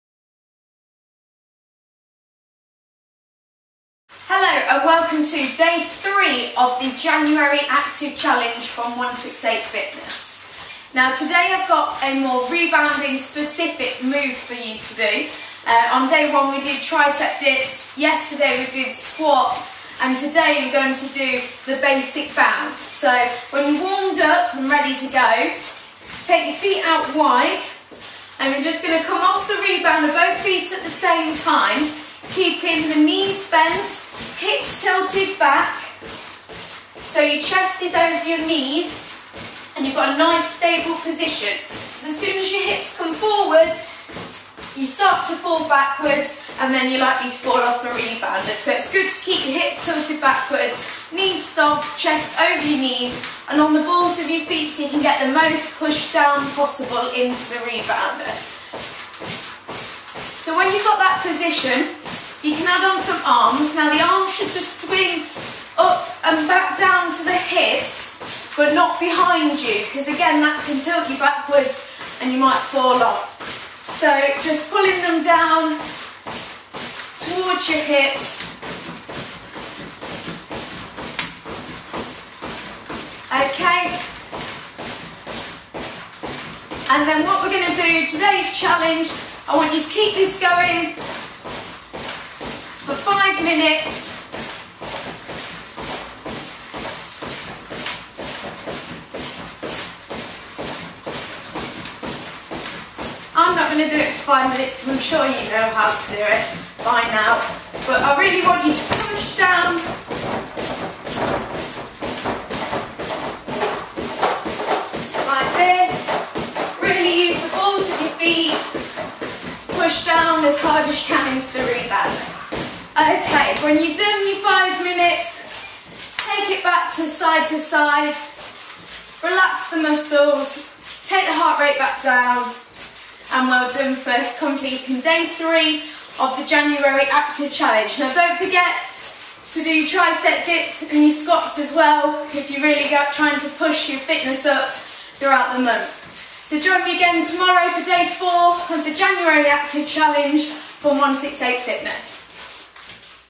Basic Bounce cardiovascular exercise on a mini trampoline
Each day you'll learn 1 of the 31 different exercises to do on an Urban Rebounding mini-trampoline. The challenge is - can you do ALL the exercises and build up your New Year fitness? For example: Day 1 you perform exercise 1 (as many times as you like) Day 2 you perform Day 2 exercise PLUS Day 1, Day 3 you perform Day 3, day 2 and Day 1 exercise etc etc until Day 31 you perform Days 31, 30, 29, 28...THE LOT! Are you up for it?
Follow the challenge and updates from other Team onesixeight: fitness members online via Twitter @onesixeightfit and on Facebook too.

See you tomorrow for exercise challenge number 4!!